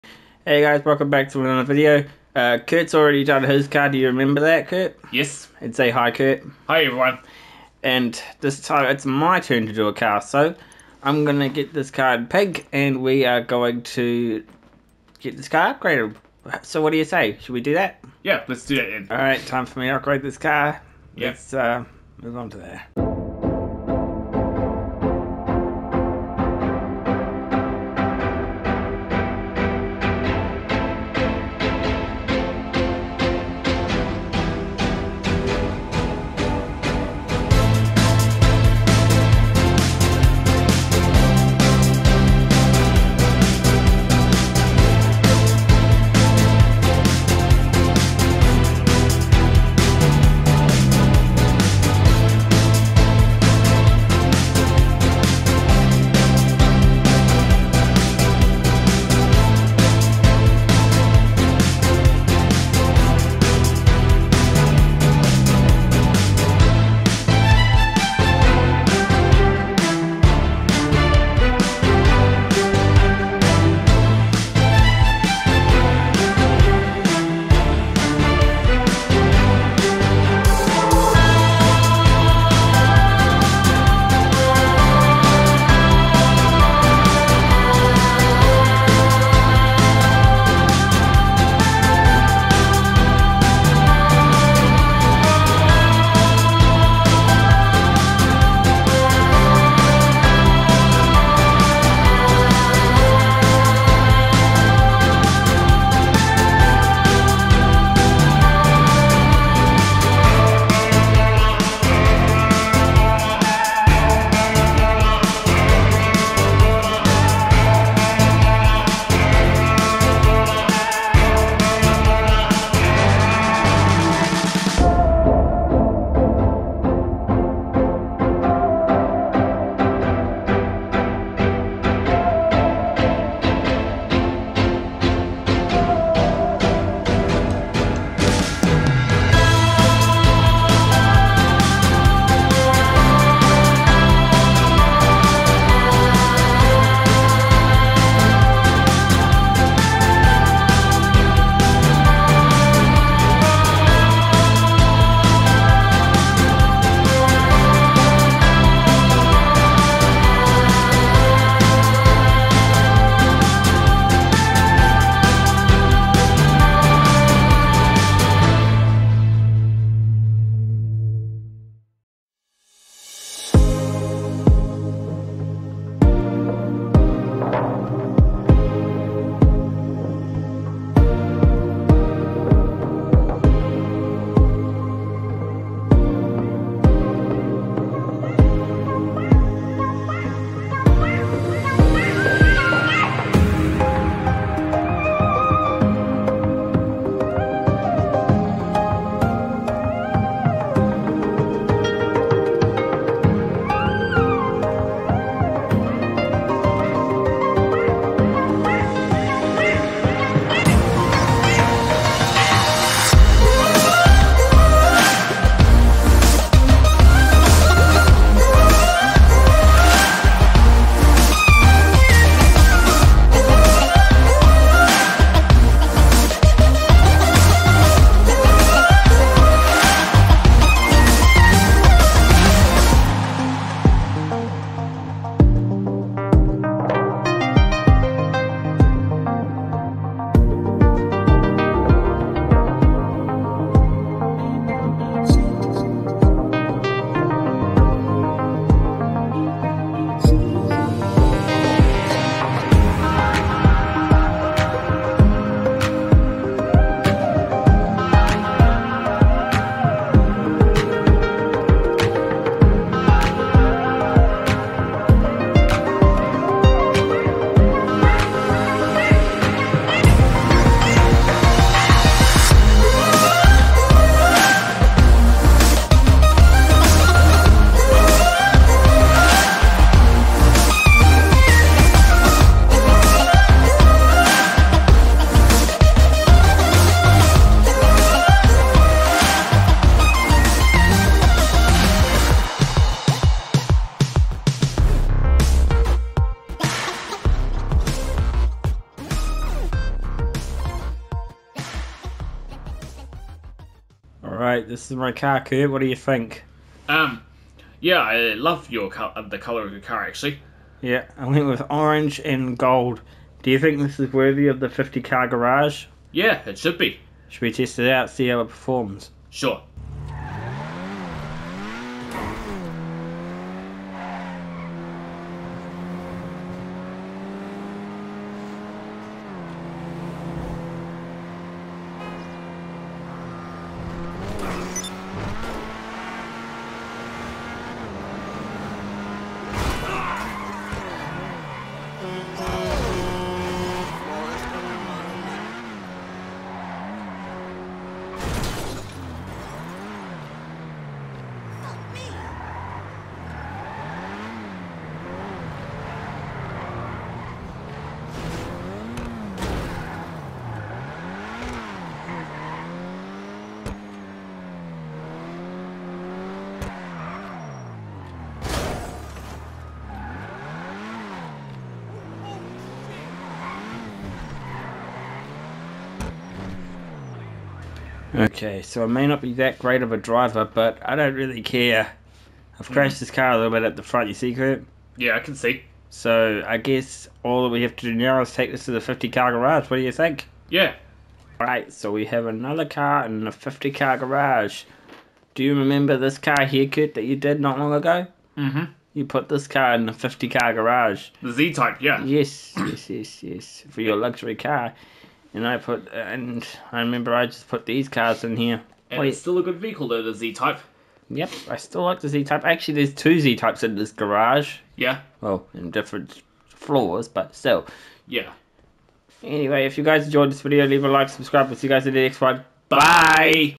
Itali RSX competition (gaming labs car)
in todays video me and my friend have a compitition to see who can make the best Itali RSX who won?

add me on discord: connorbeattie16#3738